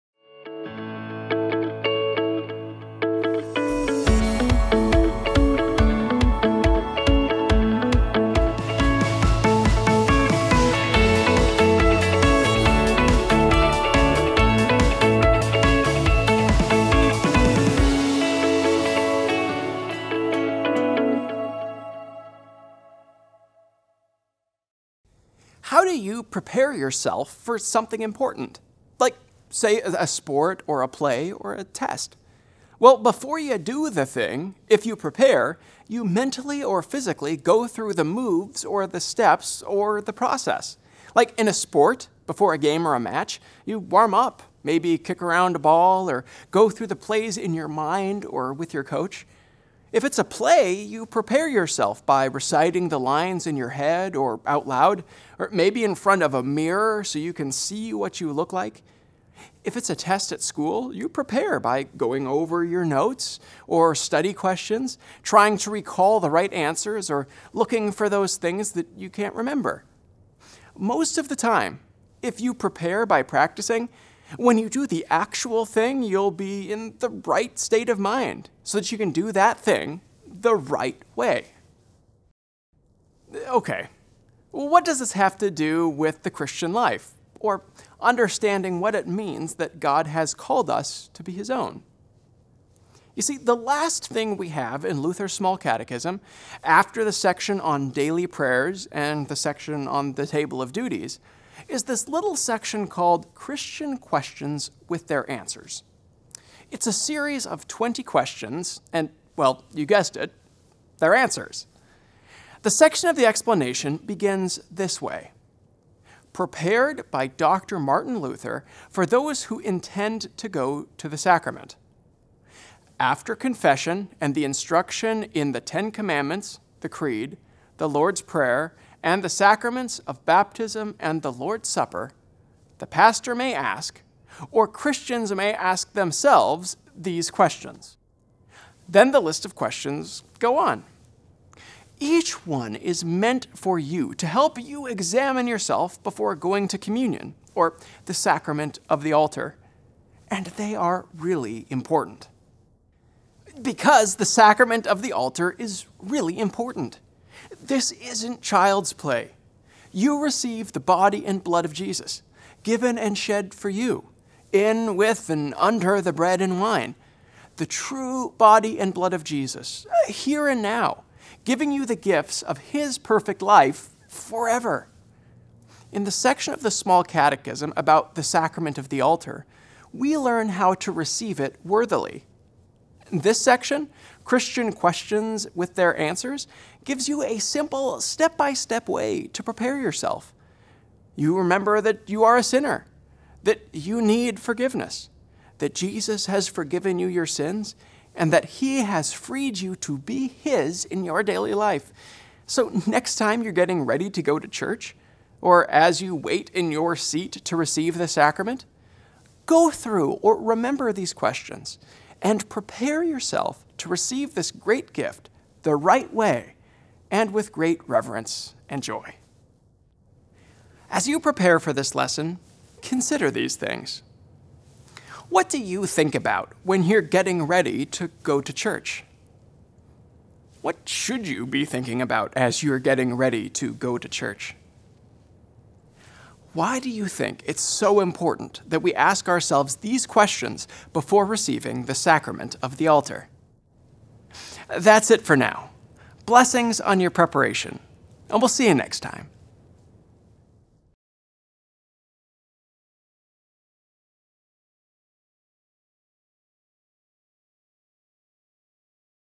Christian Questions with Their Answers
In this short video, Rev. Pete Jurchen discusses Christian questions with their answers.